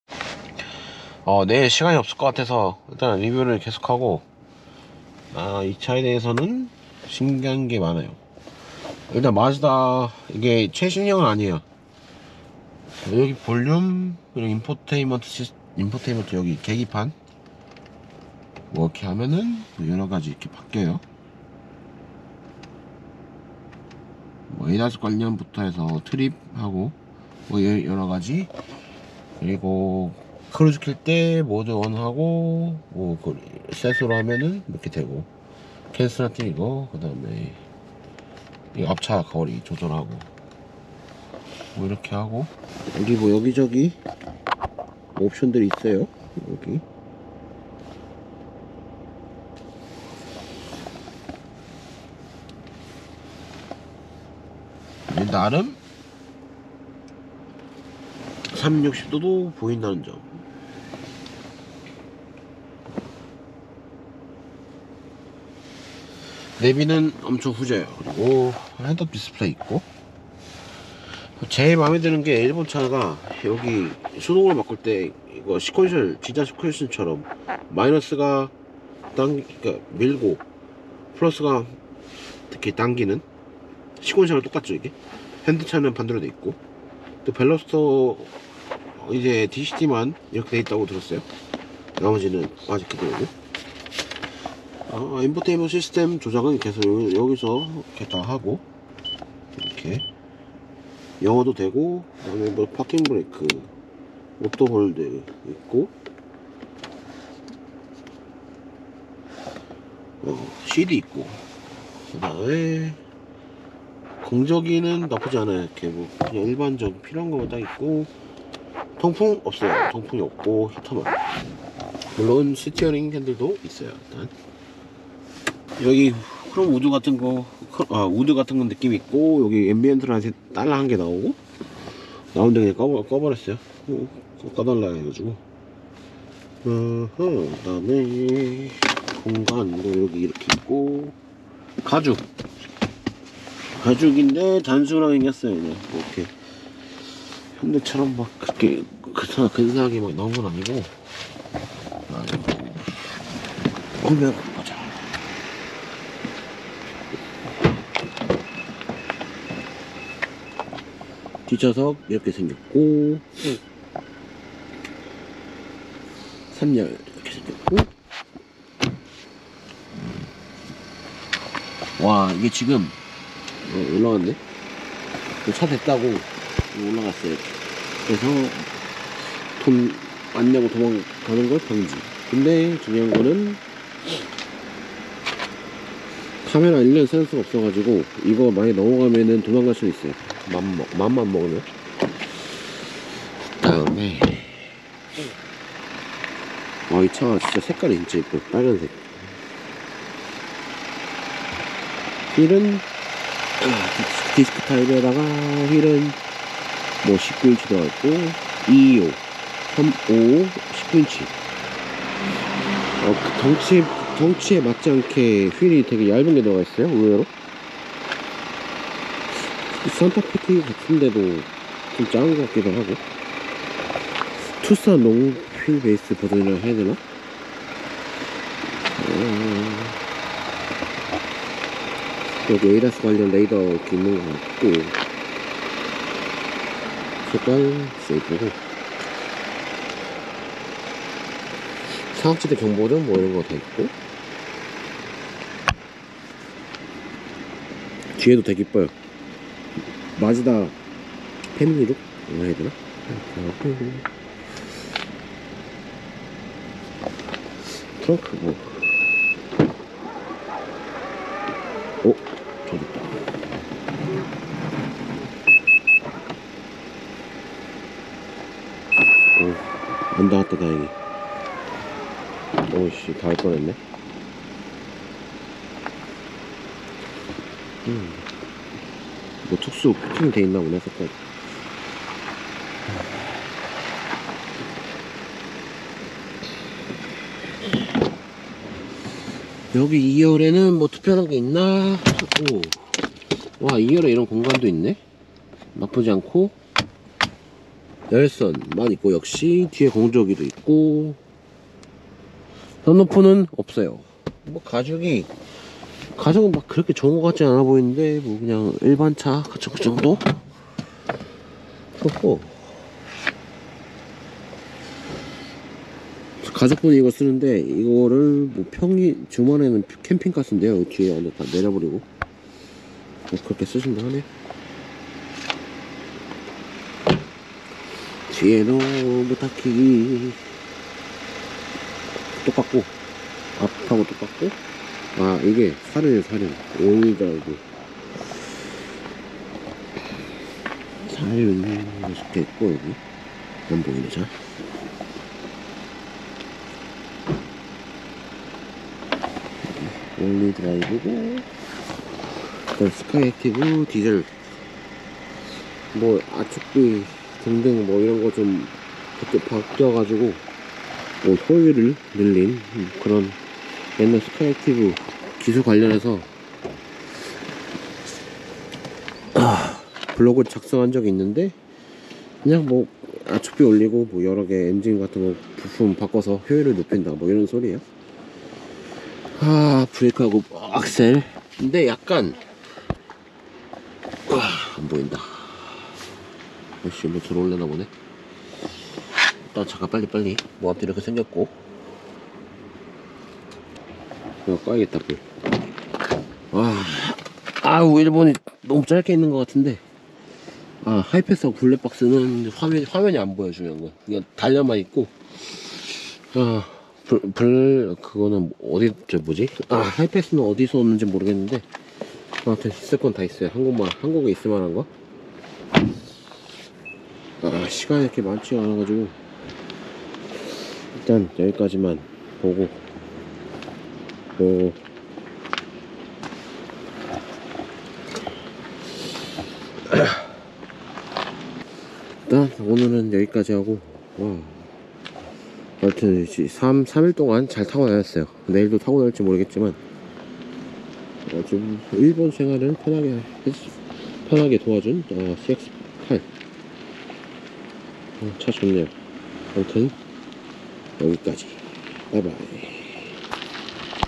마즈다 CX-8 잠깐 시승기 Mazda CX-5의 3열 시트 추가 버전 일본 자동차 문화 체험기
마쯔다 CX-8 잠깐 시승기 Mazda CX-5의 3열 시트 추가 버전 일본 자동차 문화 체험기